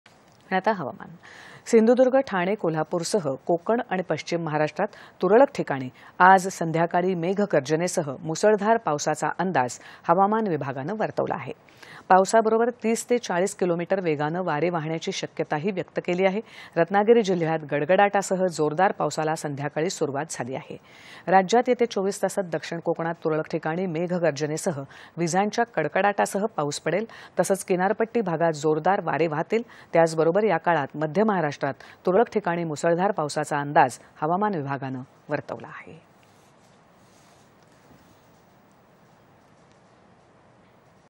हवामान स्थिति दि.०६ .११.२०२१
दि.०६ .११.२०२१ :
   सिंधुदुर्ग, ठाणे, कोल्हापूरसह कोकण आणि पश्चिम महाराष्ट्रात तुरळक ठिकाणी आज संध्याकाळी मेघगर्जनेसह मुसळधार पावसाचा अंदाज हवामान विभागानं वर्तवला आहे. पावसाबरोबर  ३० ते ४० किलोमीटर वेगानं वारे वाहण्याची शक्यताही व्यक्त केली आहे. रत्नागिरी जिल्ह्यात गडगडाटासह जोरदार पावसाला संध्याकाळी सुरुवात झाली आहे.
      राज्यात येत्या २४ तासांत दक्षिण कोकणात तुरळक ठिकाणी मेघर्गजनेसह विजांच्या कडकडाटासह पाऊस पडेल तसंच किनारपट्टी भागात जोरदार वारे वाहतील. त्याचबरोबर या काळात मध्य महाराष्ट्रात तुरळक ठिकाणी मुसळधार पावसाचा अंदाज हवामान विभागानं वर्तवला आहे.